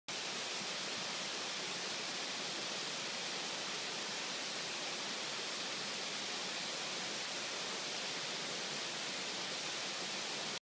Frank Lloyd Wright Falling Waters House. 11 seconds of tranquility. Enjoy!
This shot was taken of the Frank Lloyd Wright Falling Waters House on Tuesday June 11, 2013 at approx. 9:30AM.  The House was designed cica 1935 by the famous American Architect Frank Lloyd Wright for the Kaufmann Family of Pittsburgh PA as a summer retreat.

11 seconds of tranquility.  Enjoy!